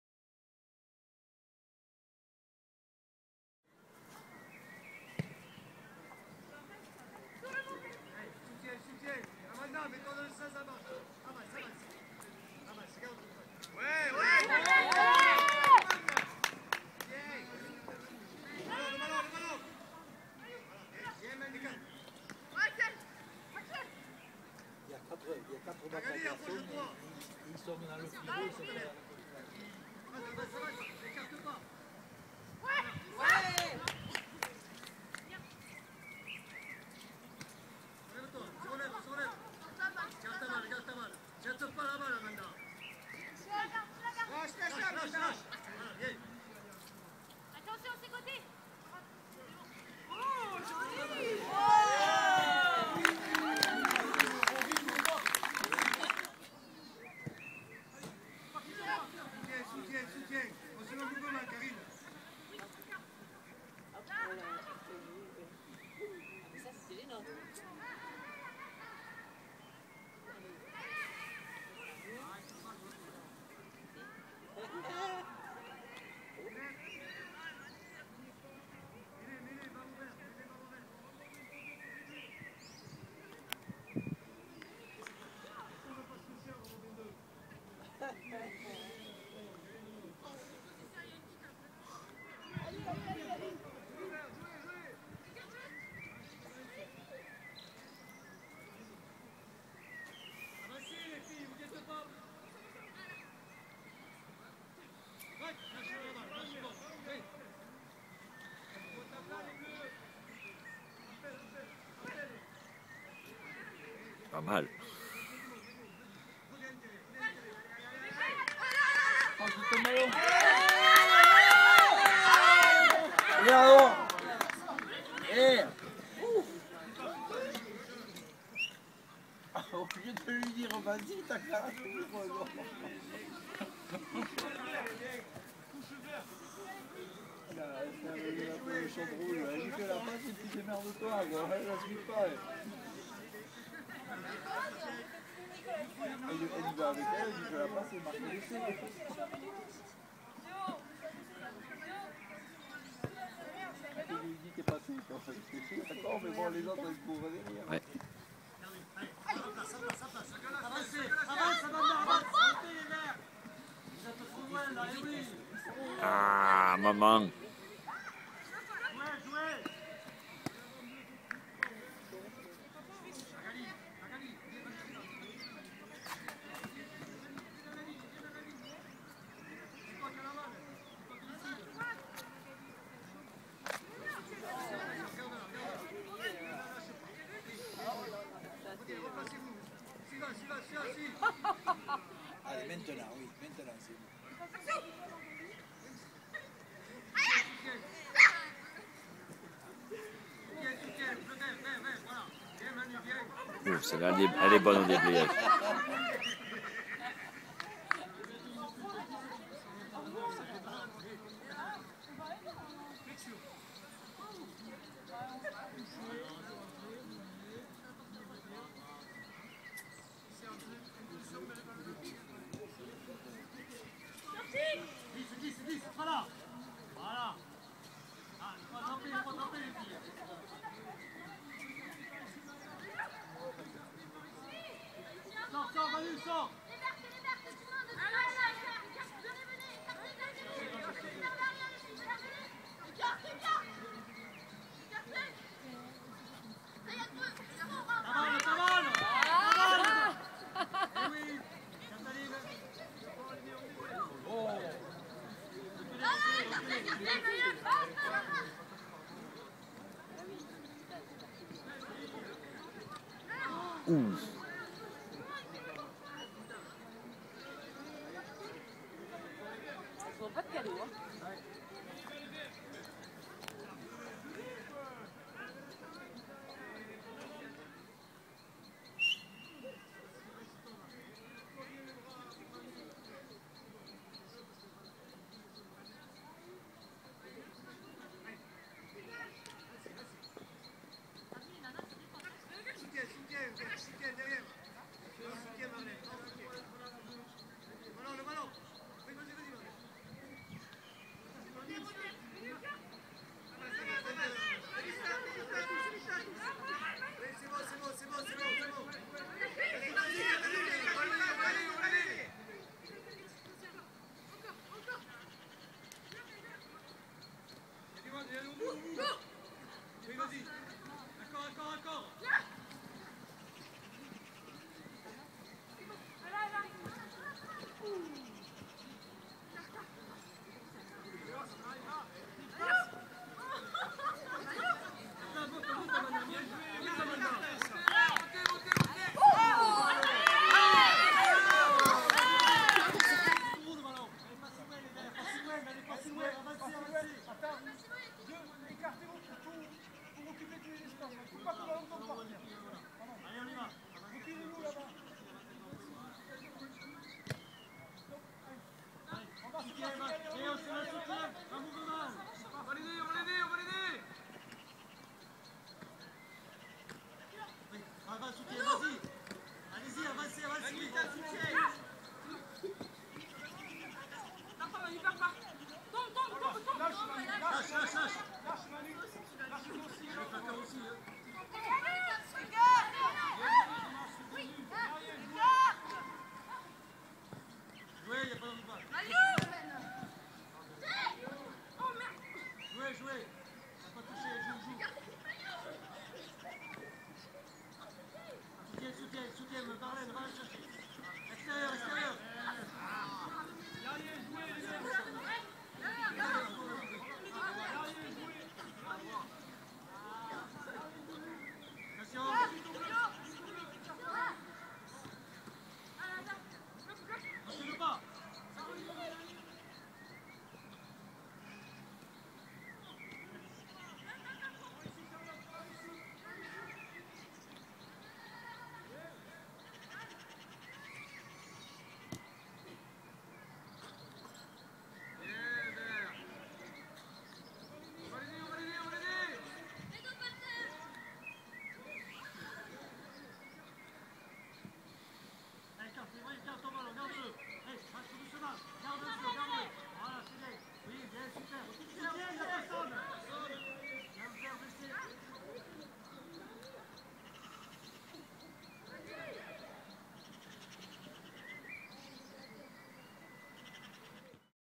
Pole confondus
Match de rugby féminin des Gilbertes vs. Pole Espoir Toulouse.